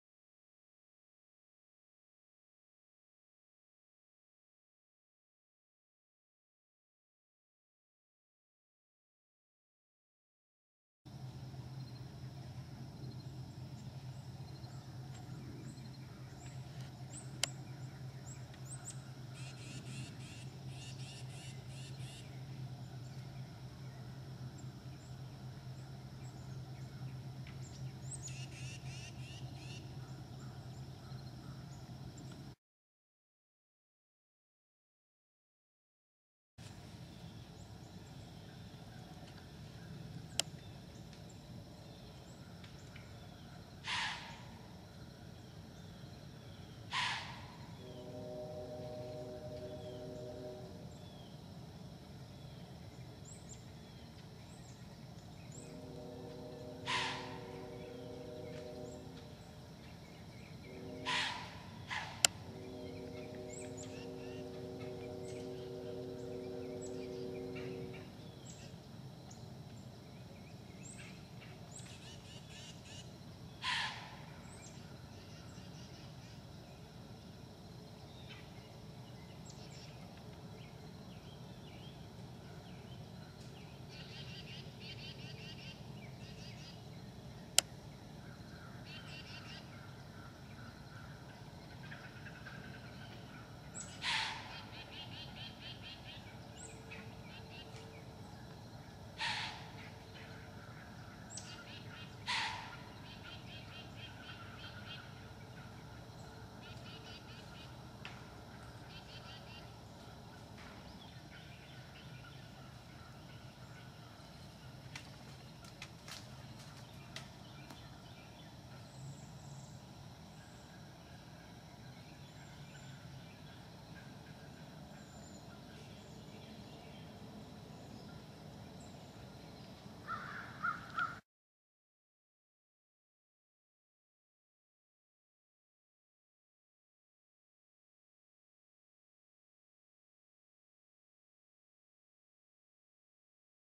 Doe Whistle 2010
Listen as does off camera whistle and snort.